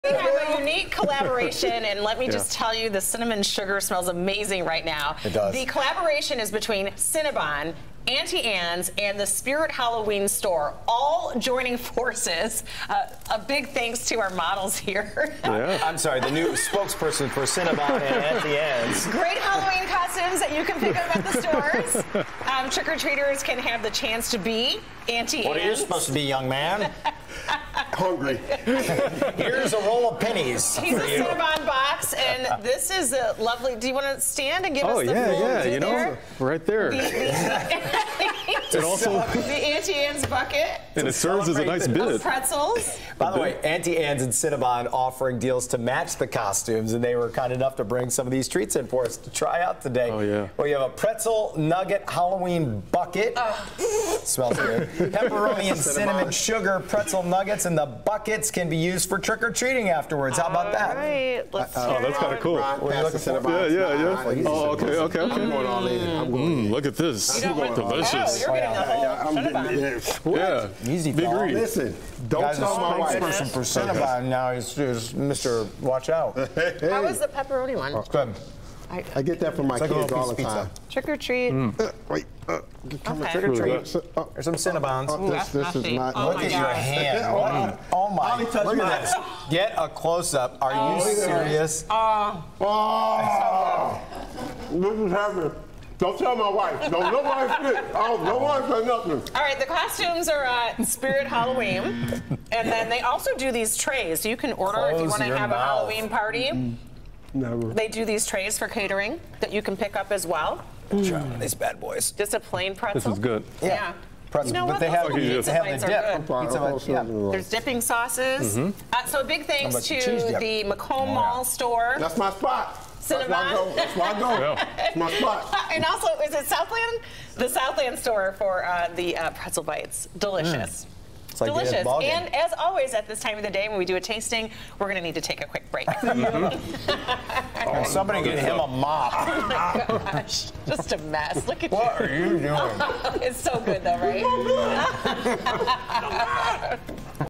Taste Test: Cinnabon & Auntie Anne's Halloween Menu & Costumes
The Local 4 Morning Show team samples the new Halloween offerings from Cinnabon & Auntie Anne's and shows off a few costume ideas.